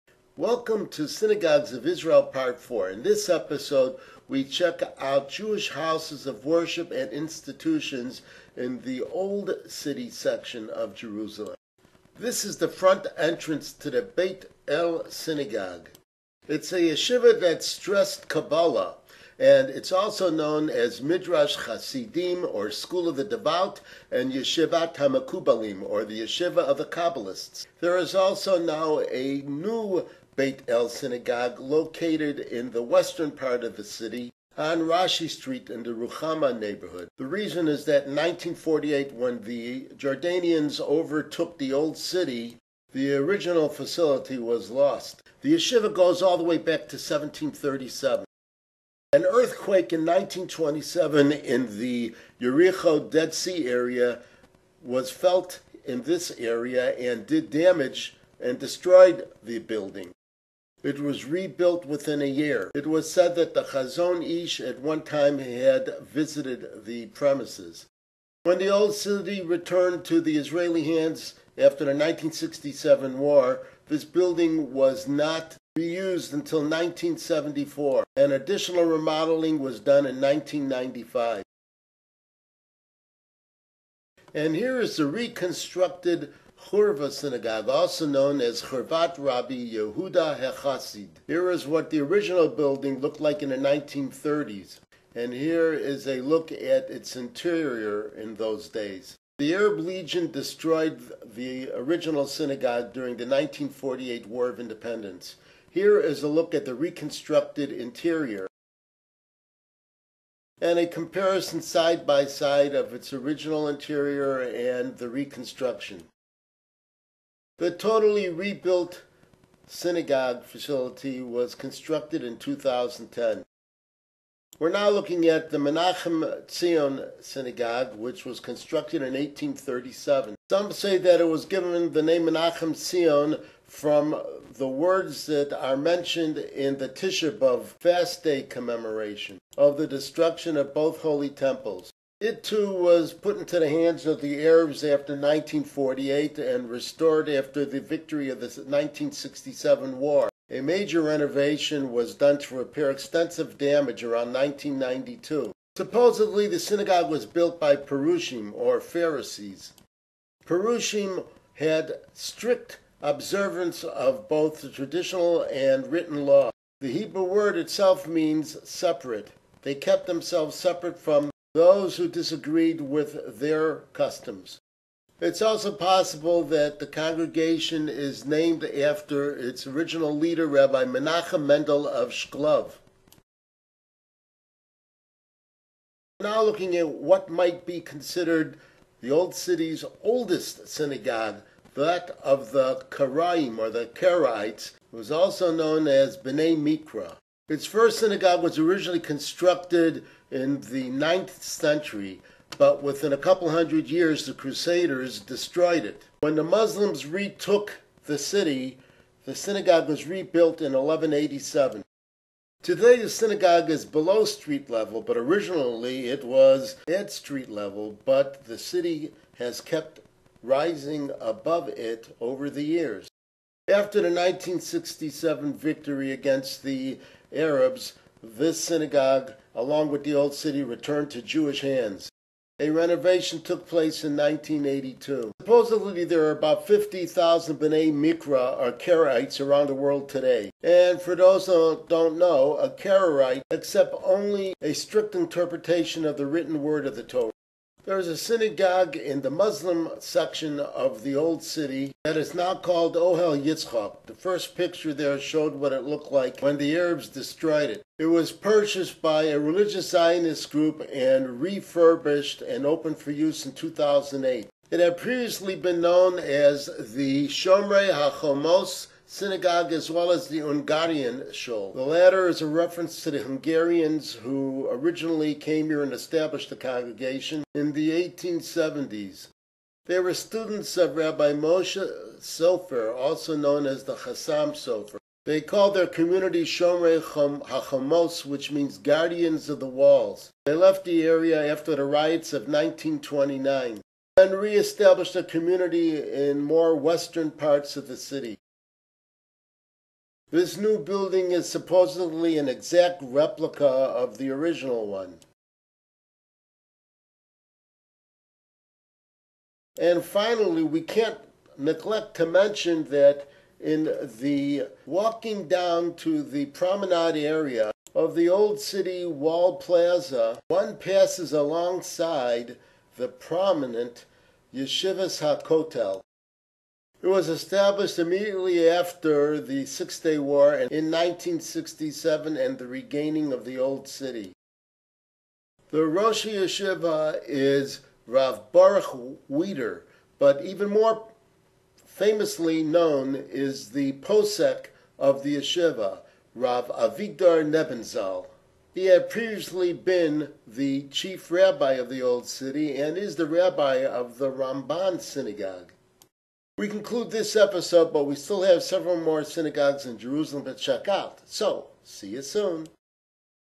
Synagogues of Israel Part 4 Jerusalem D
A fourth look at Jewish houses of worship and institutions in Jerusalem, Israel.  duration: 6 min, 3 sec.   credits: google maps, no profit intended.